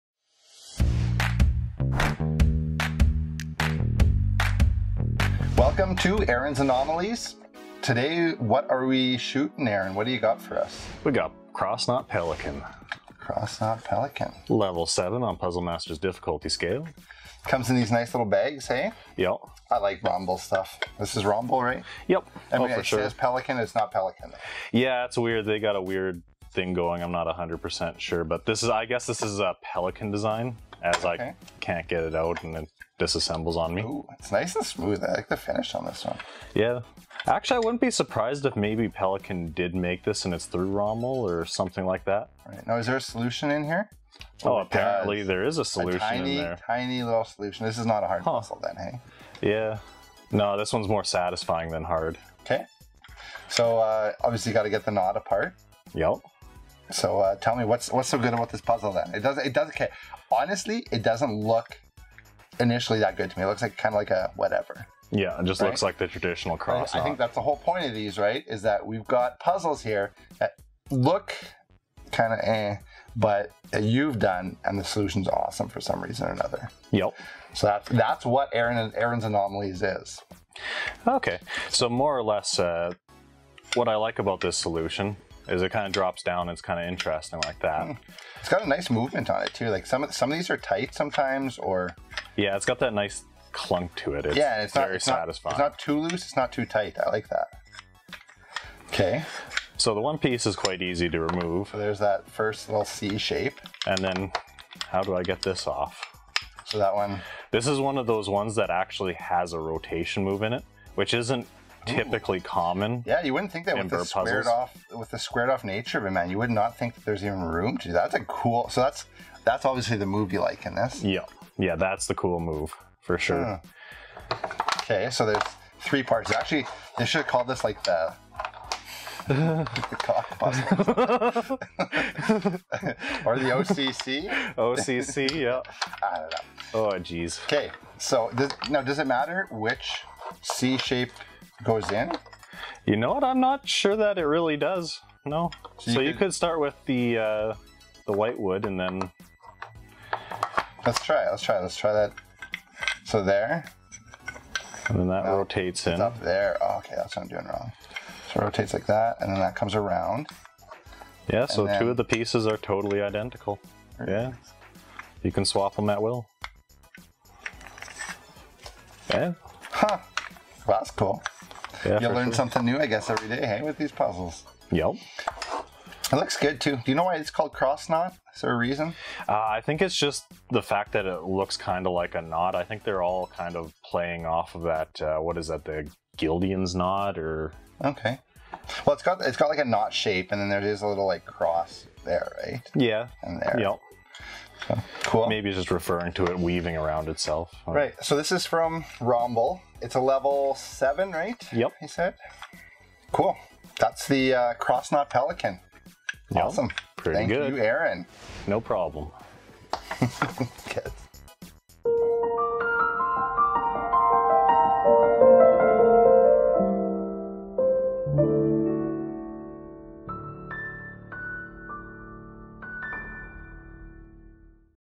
Aaron's Anomalies - Cross Knot Pelikan from Rombol
How about Cross Knot Pelikan from Rombol? Aaron's Anomalies shows you the uniqueness of this puzzle.

This puzzle consists of three pieces that appear as "rectangular" rings with cutouts.  A clever interlocking puzzle designed by Josef Pelikan. Made only with various fine fruit and deciduous trees, each game has a different wood composition (grain, color). Every puzzle is unique!